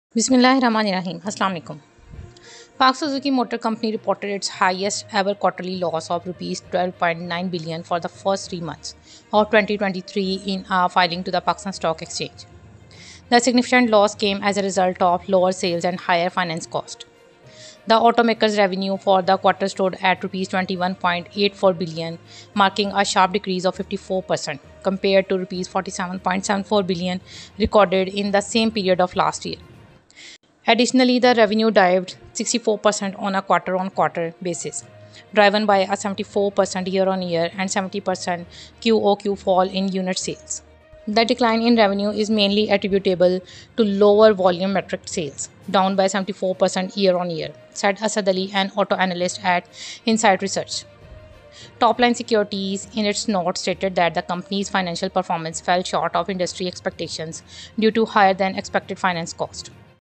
Pak Suzuki Motor reports highest quarterly loss
KARACHI:
Pak Suzuki Motor Company (PSMC) reported its highest-ever quarterly loss of Rs12.9 billion for the first three months of 2023 in a filing to the Pakistan Stock Exchange (PSX).

The significant loss came as a result of lower sales and higher finance cost.

The automaker’s revenue for the quarter stood at Rs21.84 billion, marking a sharp decrease of 54% compared to Rs47.74 billion recorded in the same period of last year.

Additionally, the revenue dived 64% on a quarter-on-quarter (QoQ) basis, driven by a 74% year-on-year (YoY) and 70% QoQ fall in unit sales.

“The decline in revenue is mainly attributable to lower volumetric sales, down by 74% year-on-year,” said Asad Ali, an auto analyst at Insight Research.